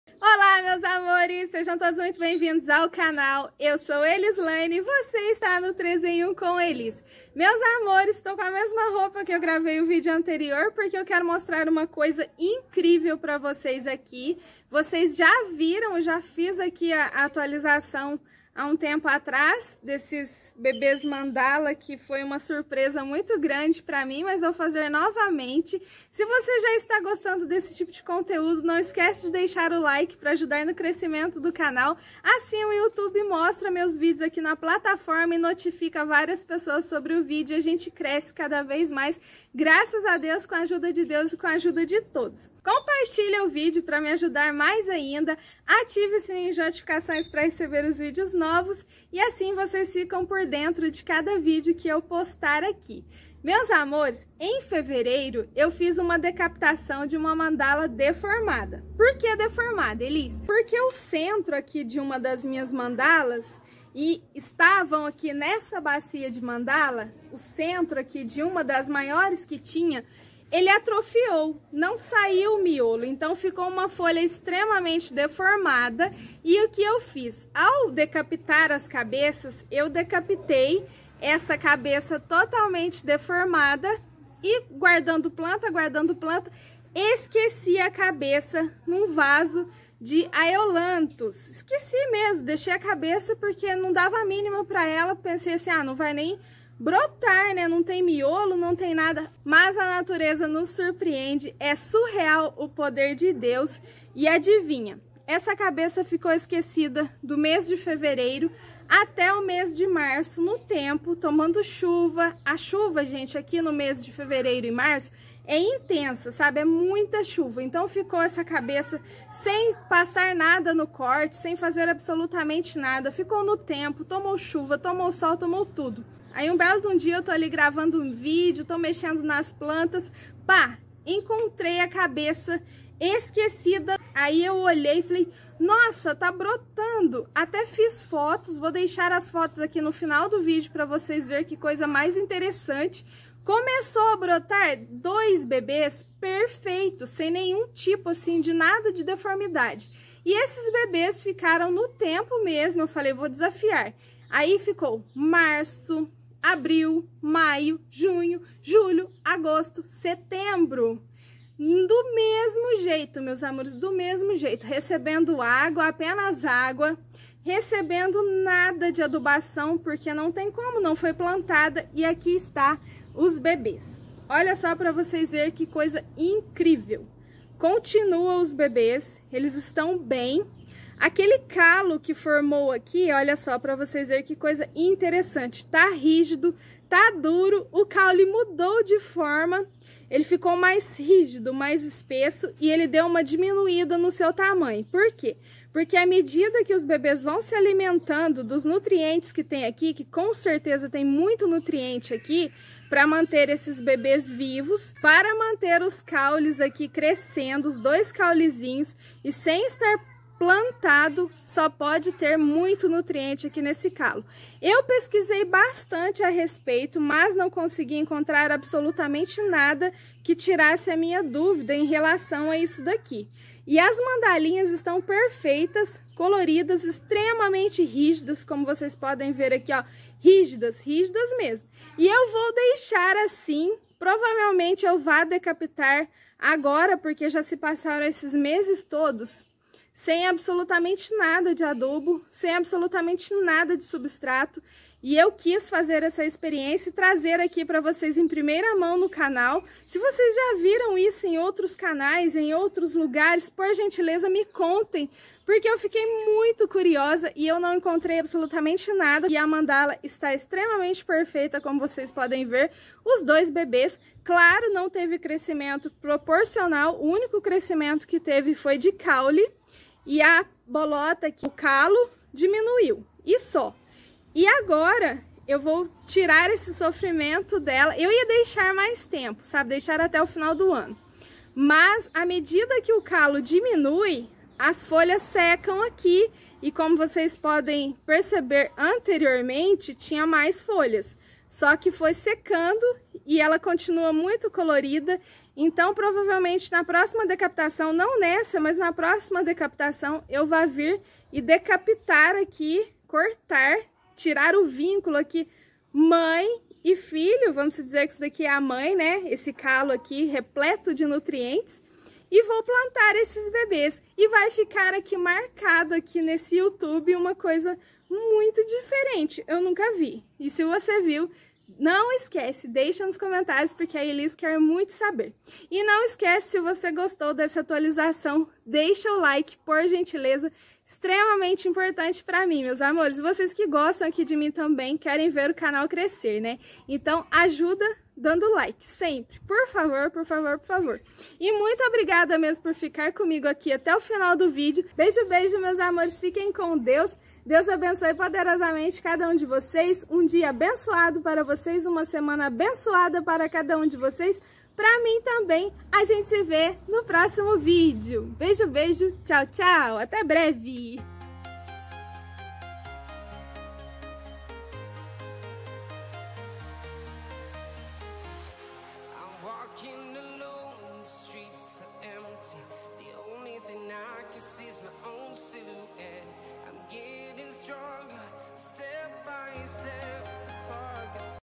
Mandala SURREAL 😱 7 meses sem substrato/ no tempo/ chuva/sol/ frio
Meus amores, nesse vídeo mostro para vocês mandalas bicéfalas, protegidas por um "calo " . Sobrevivendo sem substrato a 7 meses no tempo. Sobreviveu a chuva, ao sol, geada , frio entre outras adversidades.Nunca vi isso na minha vida😳